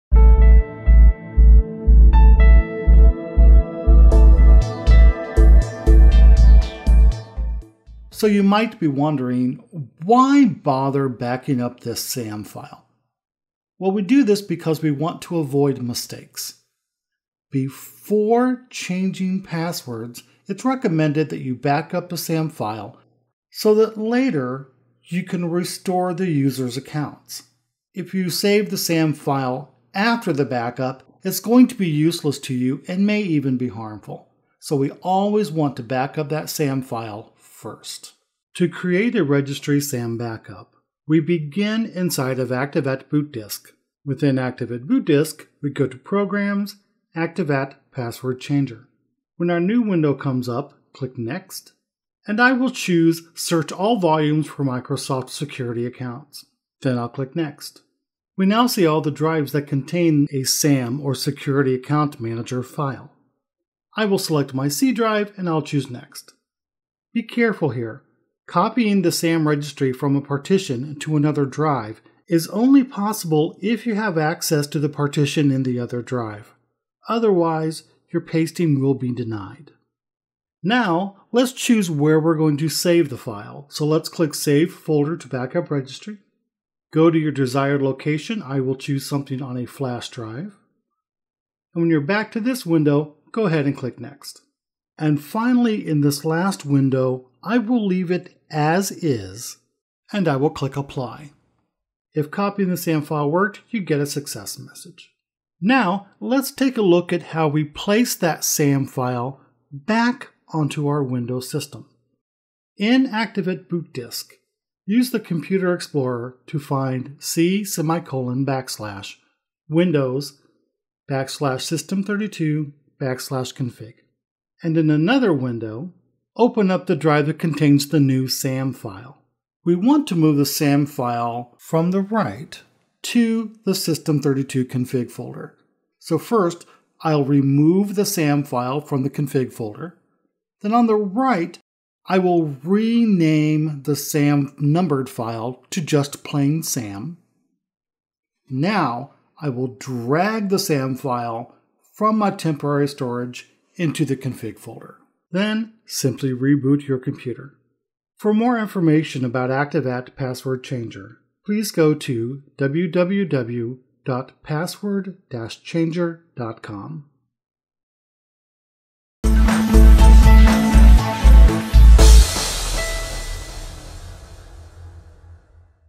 Active@ Password Changer. Registry SAM backup
If your system became inoperable after you use Active@ Password Changer you can roll back all changes to the square one.